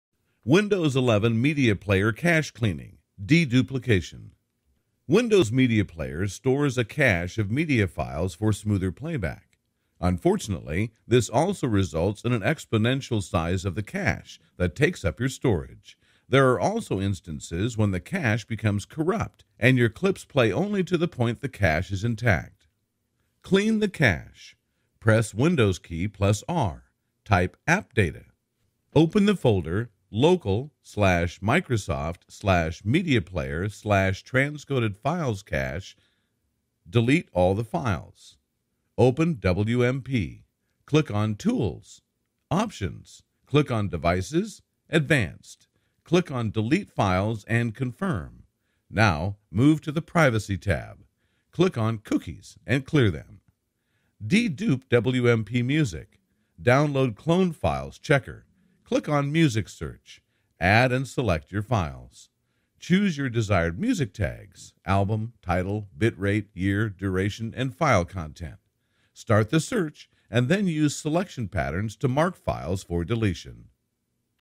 Windows 11 Media Player Cache Cleaning _ Deduplication
Windows Media Player stores a cache of media files for smoother playback.
Unfortunately, this also results in an exponential size of the cache that takes up your storage.
There are also instances when the cache becomes corrupt and your clips play only to the point the cache is intact.

Clean the Cache
Press Windows Key + R.
Type “appdata”
Open the folder:
..\Local\Microsoft\Media Player\Transcoded Files Cache
Delete all the files.
Open WMP.
Click on Tools (Options).
Click on Devices ( Advanced).
Click on Delete Files and confirm.
Now, move to the Privacy tab.
Click on Cookies and clear them.

Dedupe WMP Music
Download Clone Files Checker.
Click on Music Search Add and select your files.
Choose your desired music tags (album, title, bit rate, year, duration and file content).
Start the search and then use selection patterns to mark files for deletion.